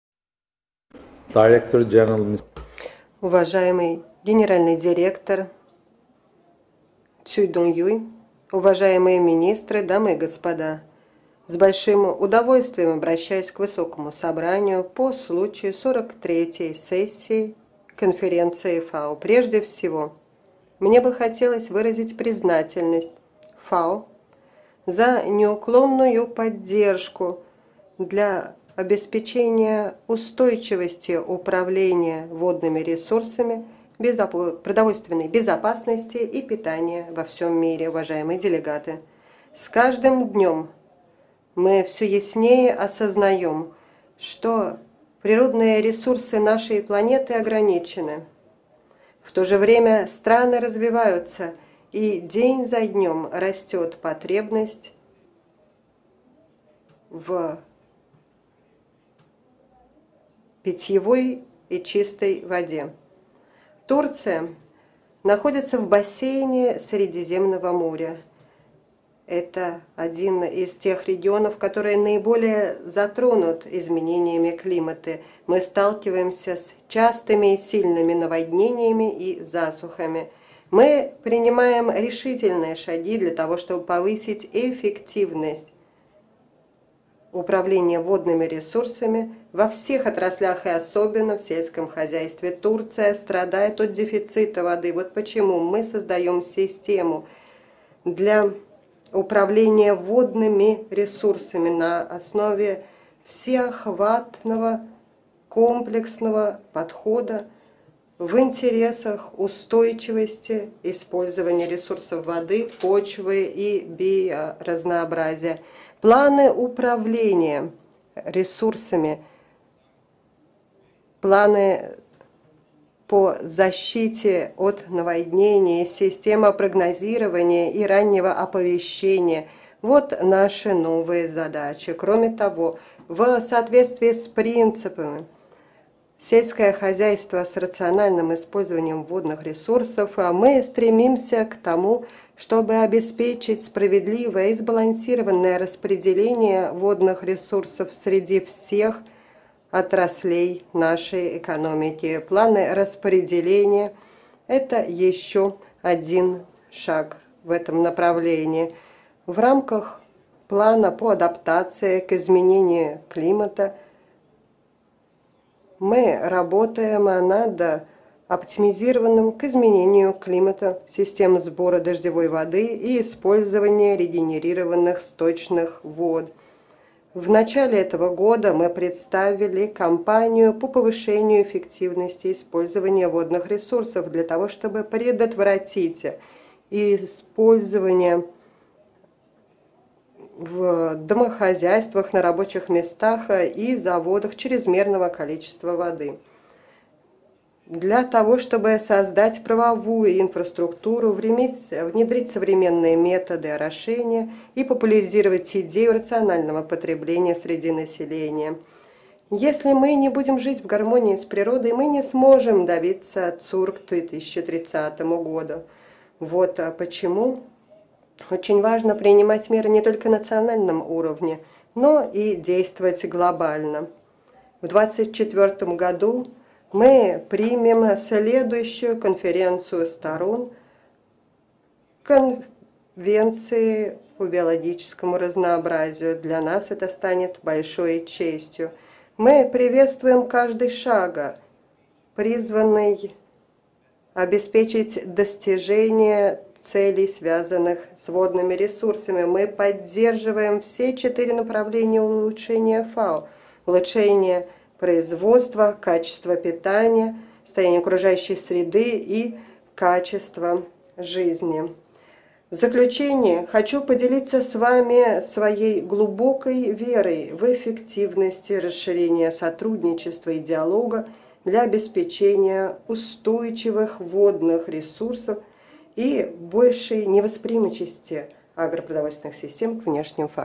C43 Заявление конференции, пункт 10 - Турция
C43 Conference Statement Item 10 -  H.E. İbrahim Yumaklı, Minister for Agriculture and Forestry of the Republic of Türkiye